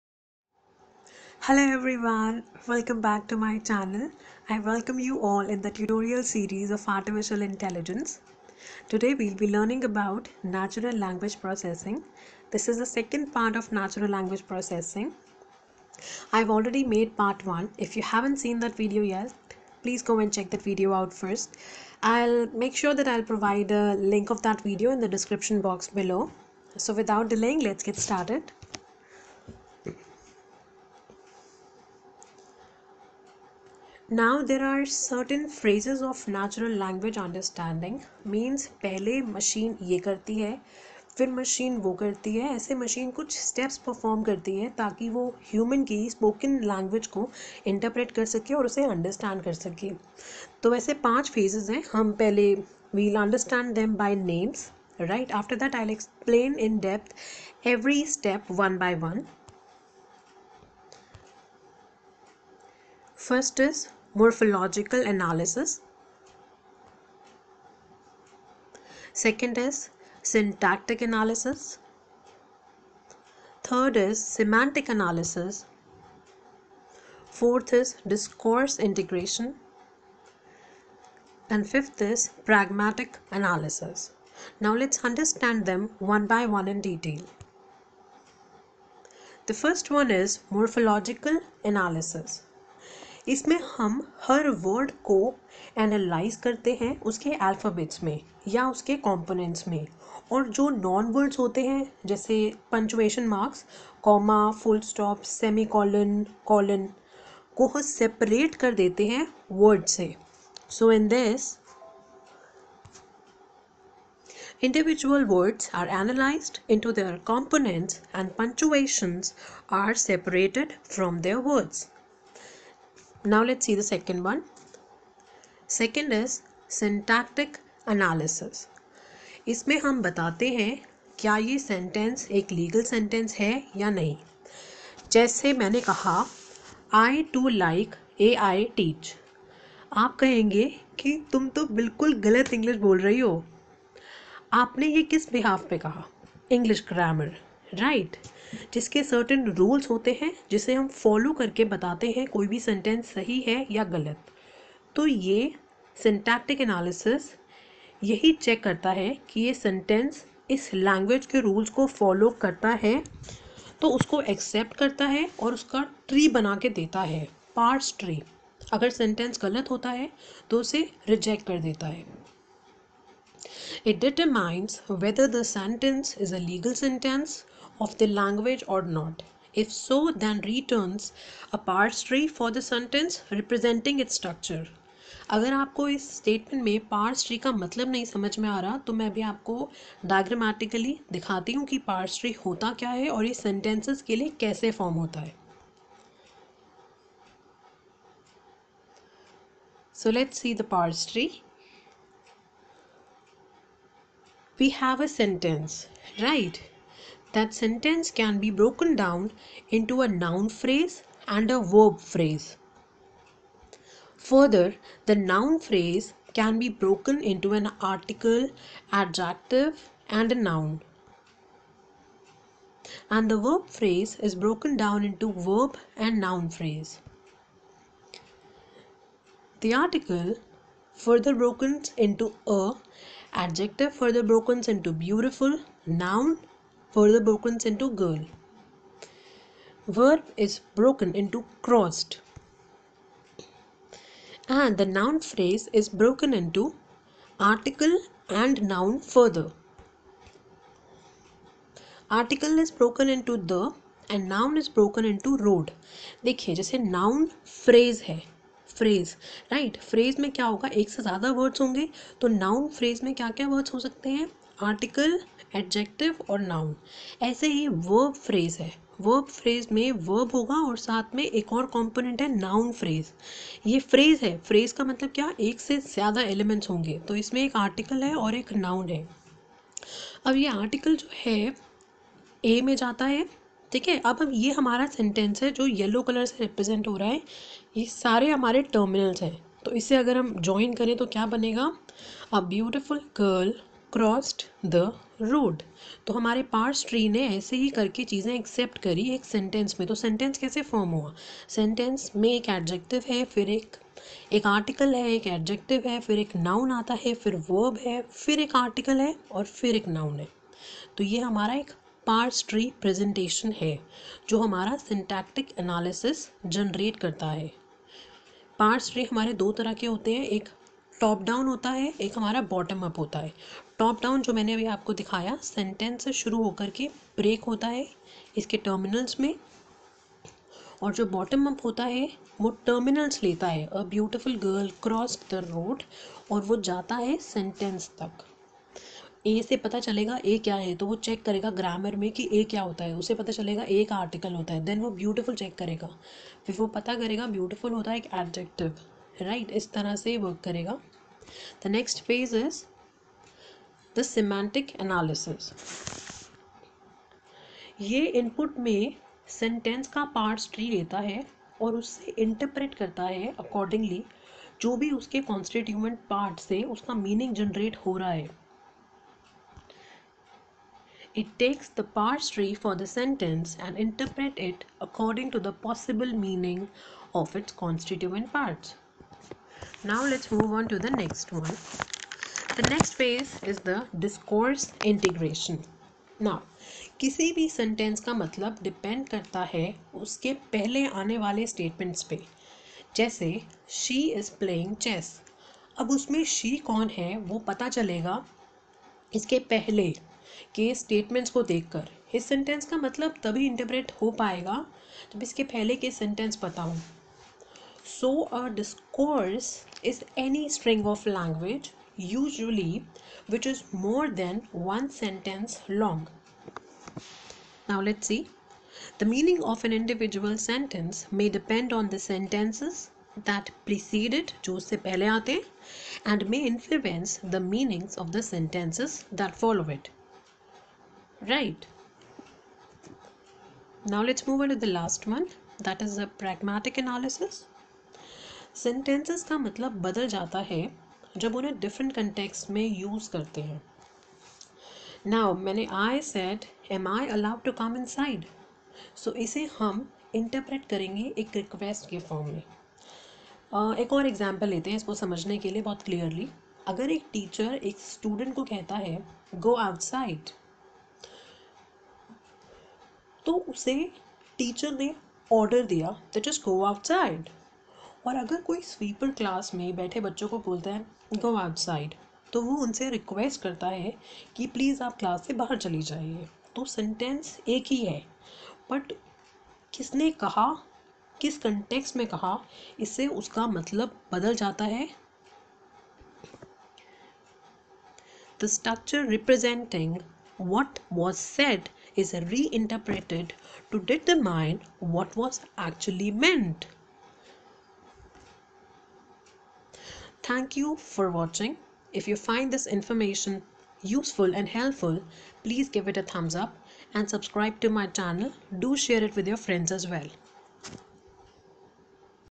Natural Language Processing (Part 2) | Artificial Intelligence series | English - Hindi | #5
This is the tutorial on natural language processing.It contains the phases of natural language understanding which are as follows :-
1. Morphological Analysis
2. Syntactic Analysis explanation with PARSING TREE
3. Semantic Analysis
4. Discourse Analysis
5. Pragmatic Analysis